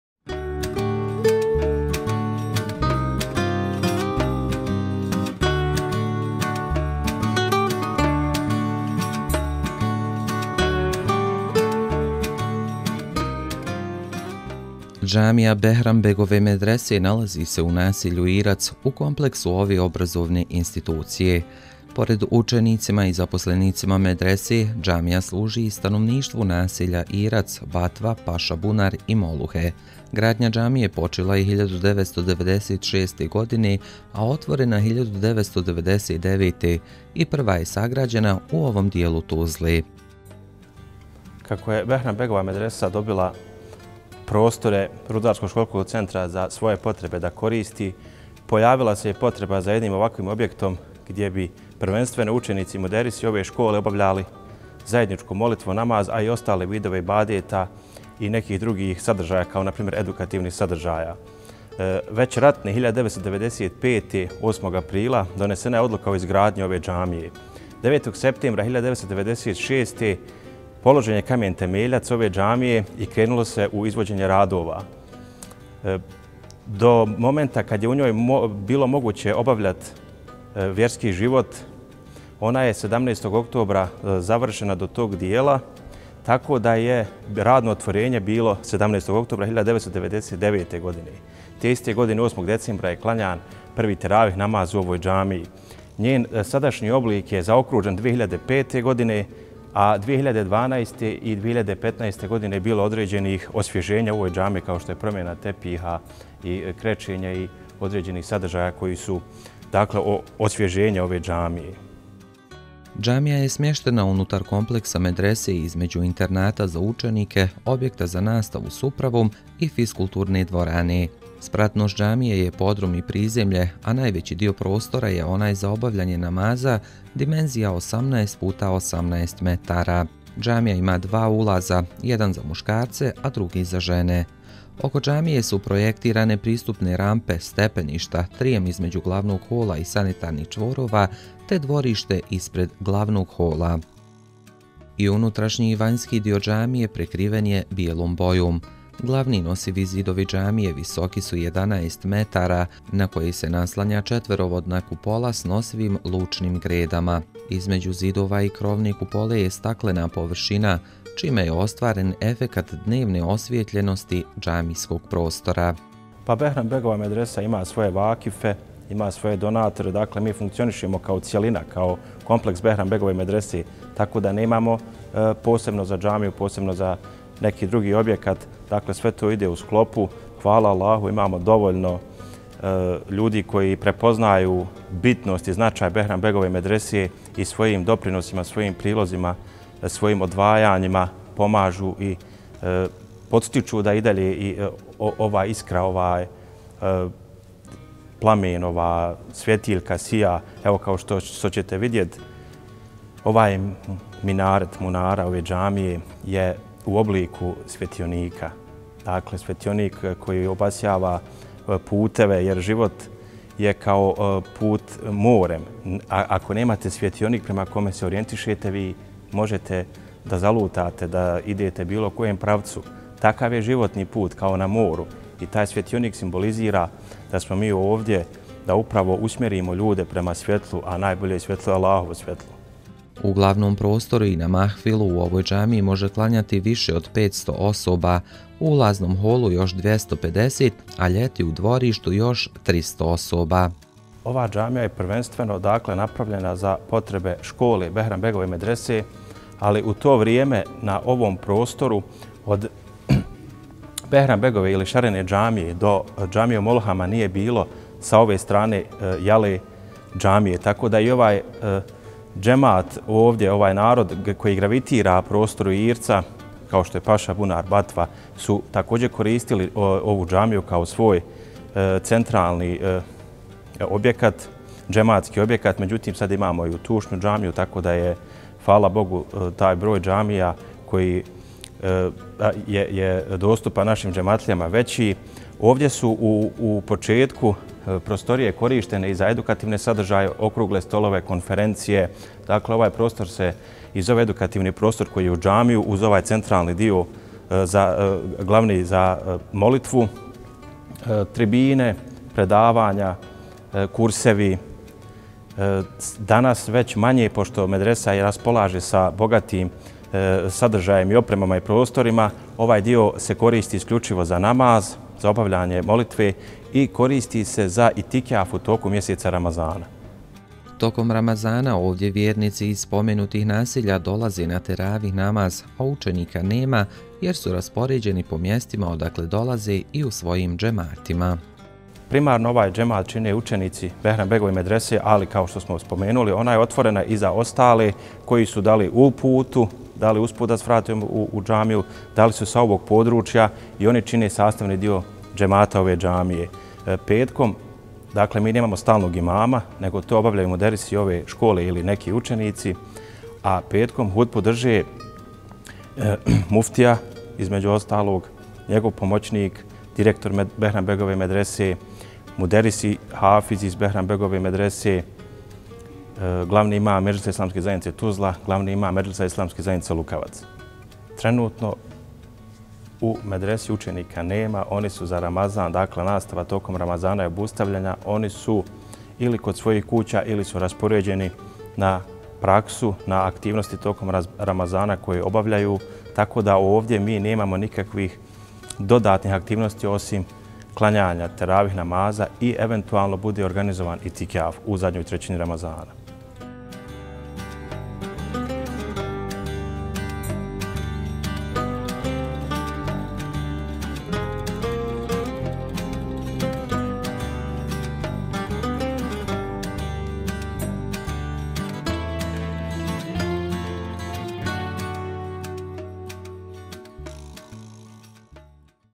Tuzlanski vjerski objekti – Džamija Behram-begove medrese
Džamija Behram-begove medrese nalazi se u naselju Irac, u kompleksu ove obrazovne institucije. Gradnja džamije počela je 1996. godine, a otvorena 1999., i prva je sagrađena u ovom dijelu Tuzle. Tokom Ramazana ovdje vjernici iz nekoliko naselja dolaze na teravih namaz, a učenika nema, jer su raspoređeni po mjestima odakle dolaze i u svojim džematima.